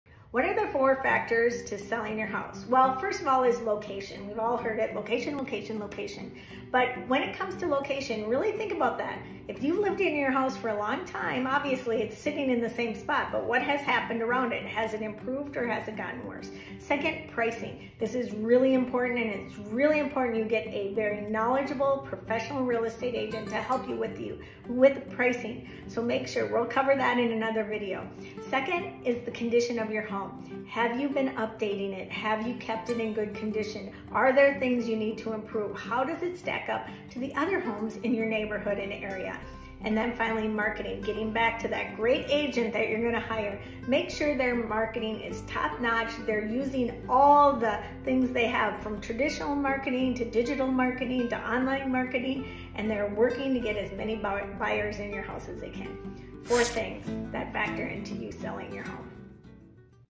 Four Factors that SELL Your House:
QUESTION FOR YOU...which one of these factors stresses you out the most?!
📌location
📌pricing
📌condition of the home
📌marketing
The right agent will guide you to address all four areas. OK, maybe we can't do anything about location, but we can help with the rest! Want to hear all the steps I take to list your home? Reach out.